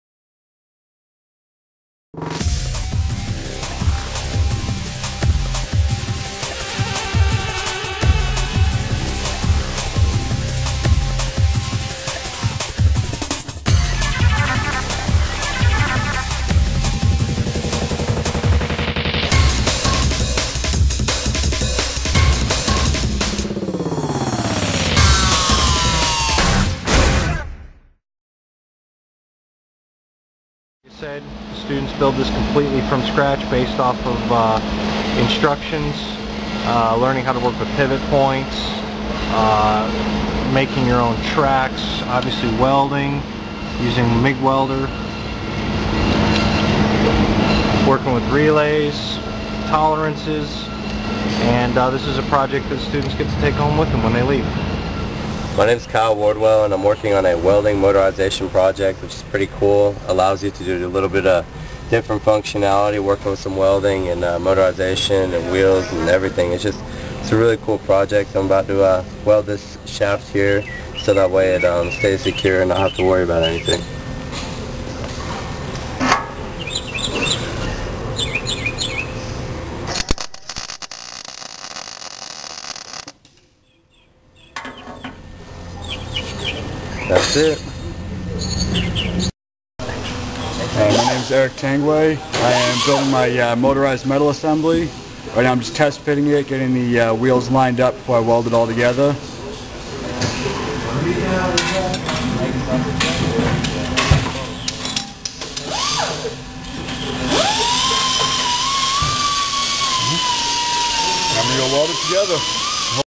Welding and Motorization projects
Students show off the welding and motorization project.
for more information.